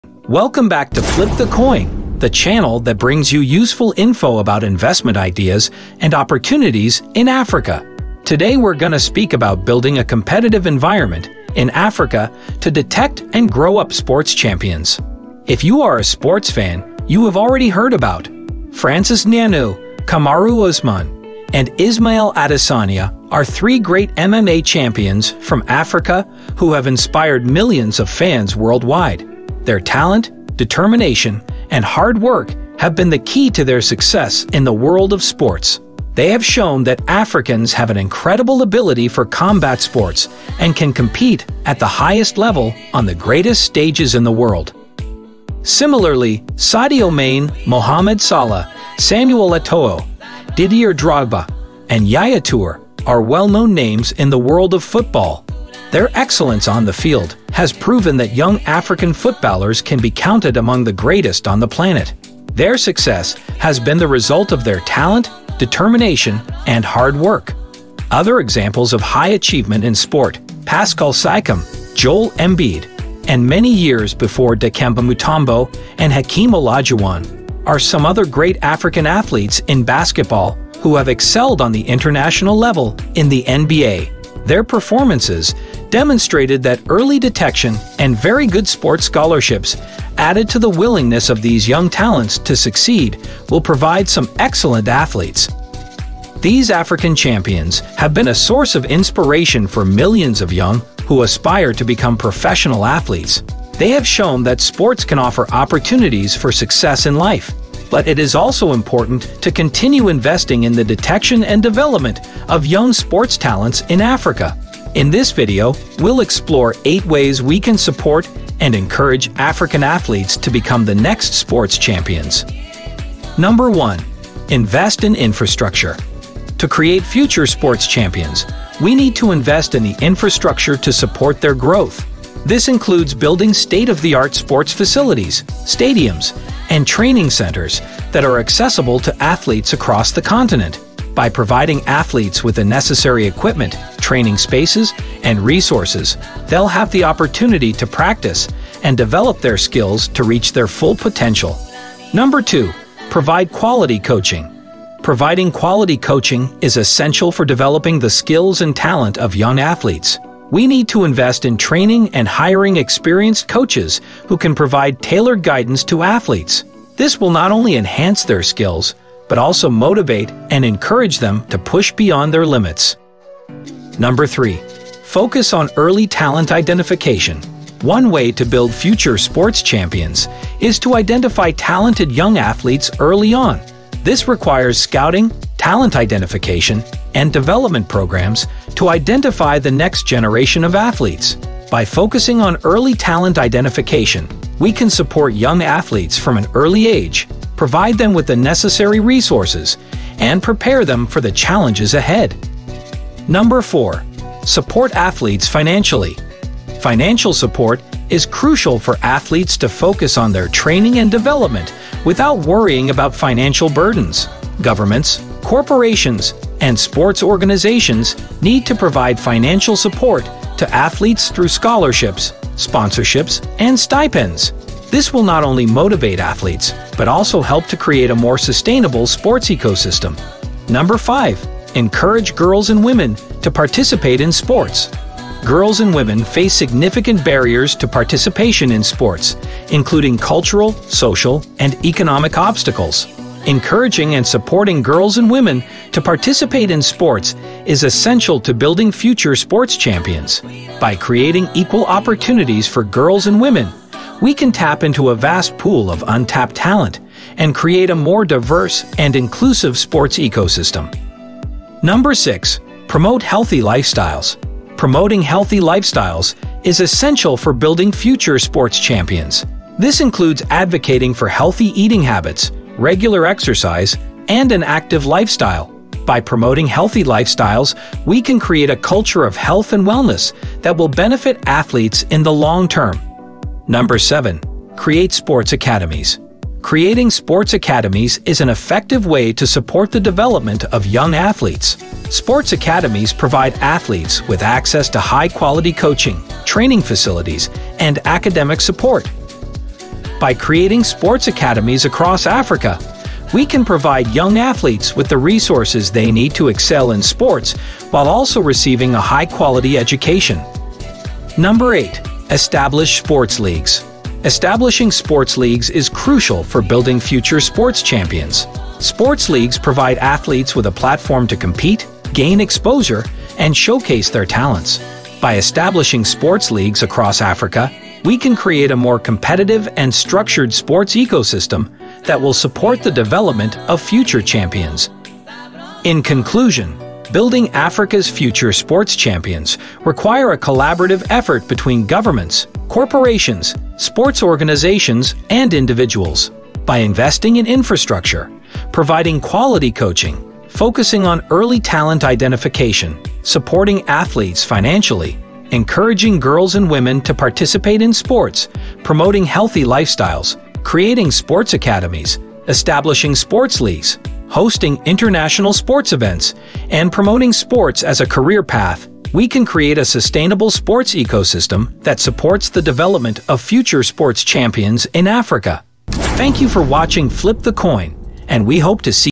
Africa's Undiscovered Sport Championship: A Hidden Opportunity for Investing?
Africa's Undiscovered Sport Championship is a hidden opportunity for investment. In this video, we'll discuss why the tournament could be a profitable investment and why now may be the best time to invest.

Africa's Undiscovered Sport Championship is an event that has the potential to be a huge financial success. We'll discuss the reasons why and why now may be the best time to invest in the tournament. If you're interested in investing in the tournament, be sure to watch this video to learn more!